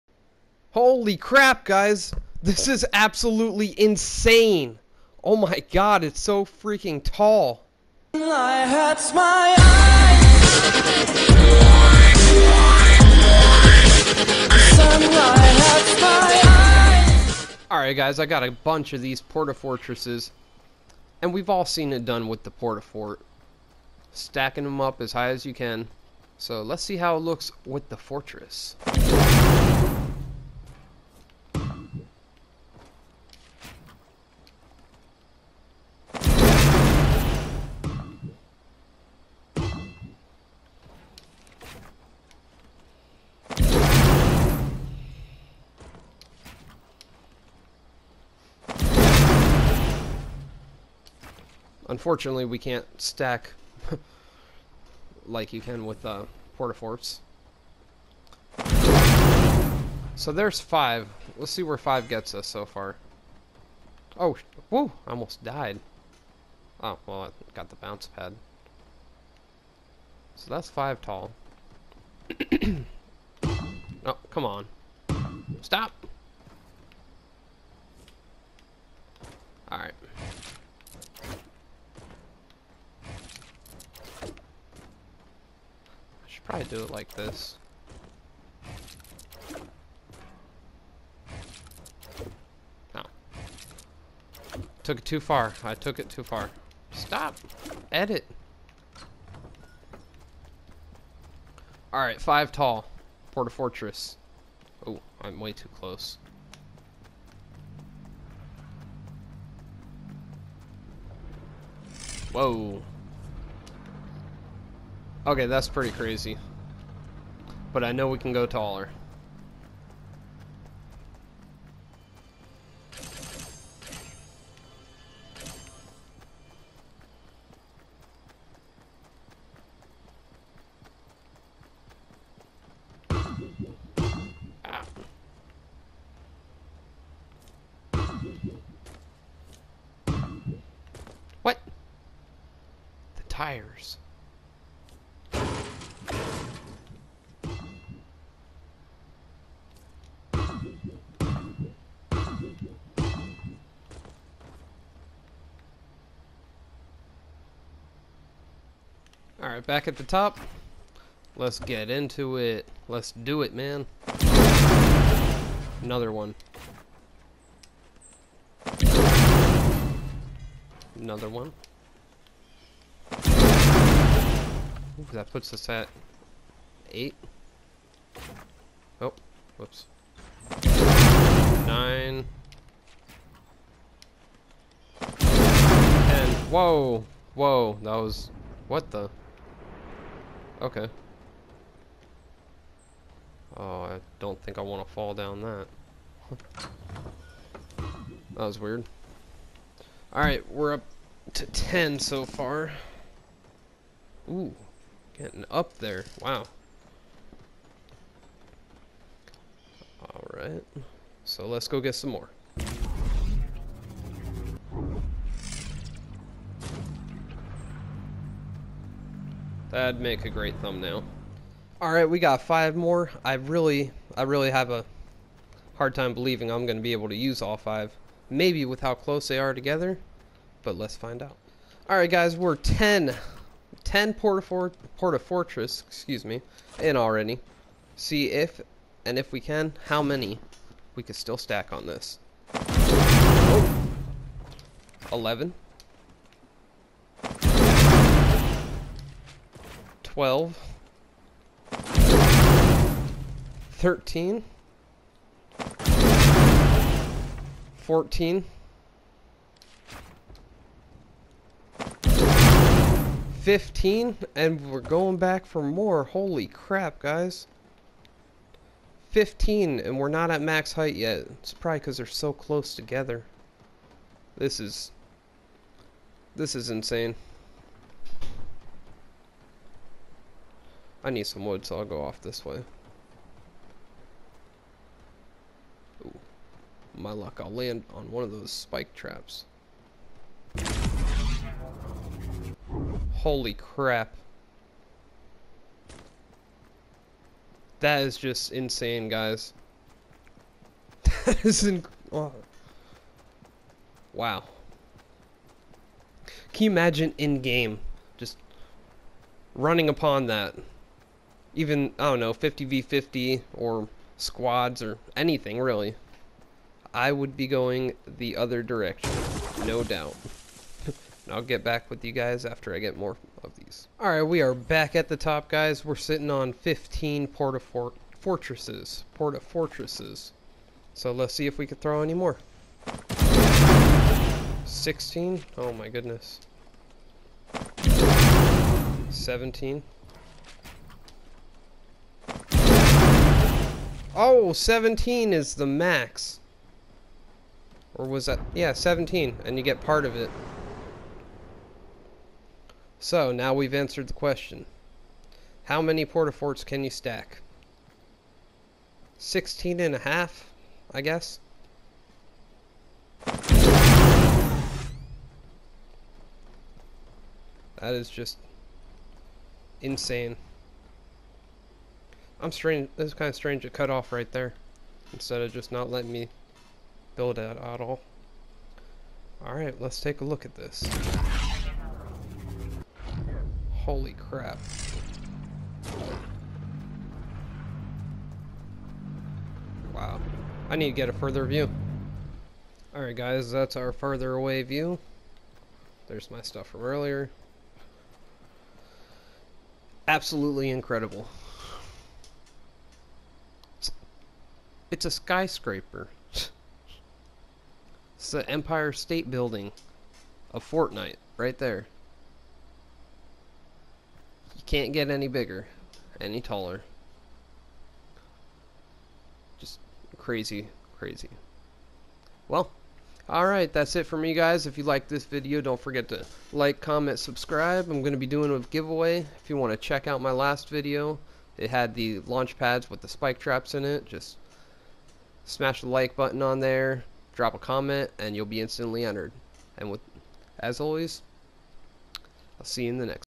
Port-a-Fortress at *Max Hight* *New item*
I tested the exact number and height of the new port-a-fortress!!!